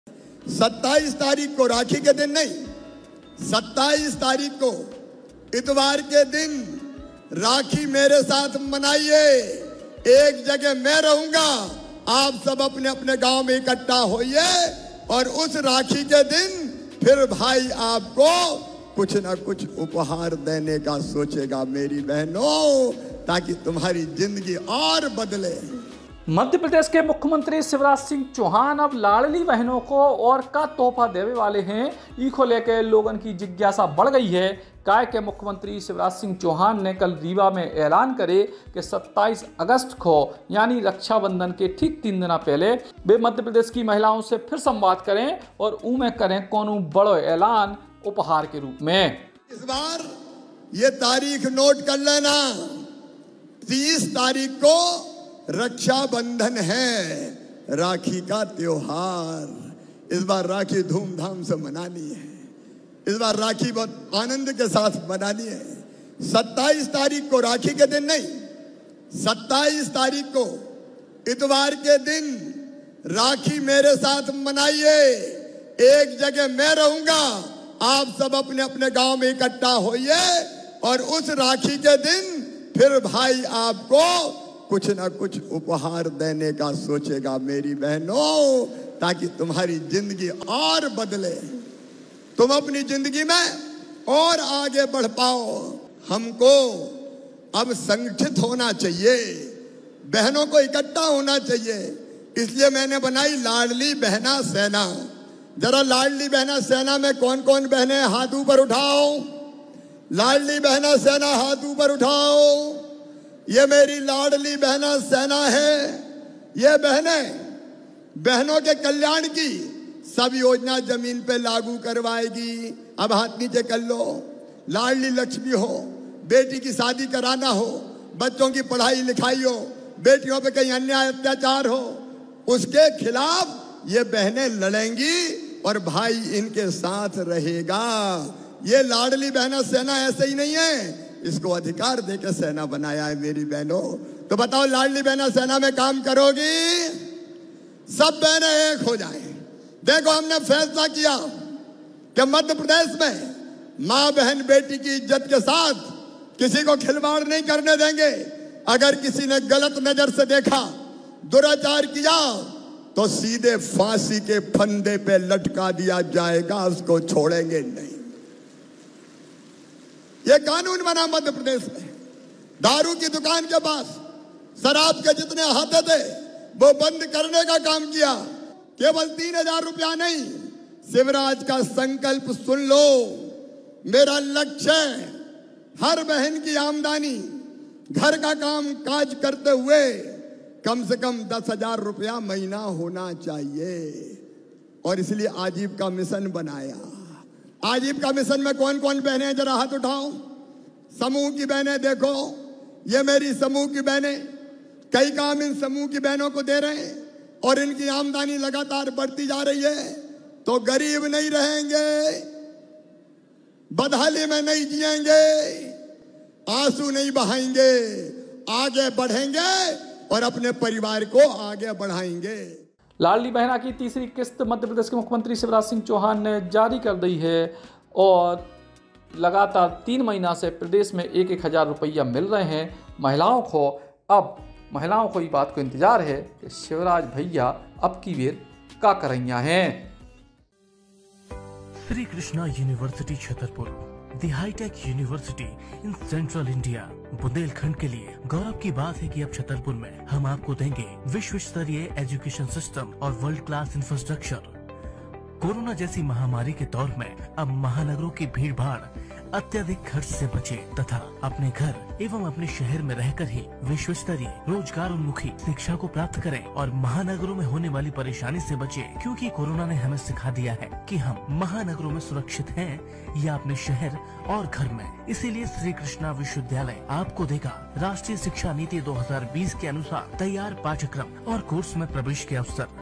CM शिवराज लाड़ली बहनों को देंगे एक और तोहफा, सुनिए ये ऐलान
BUNDELI BAUCHAAR

Suggested Pages :-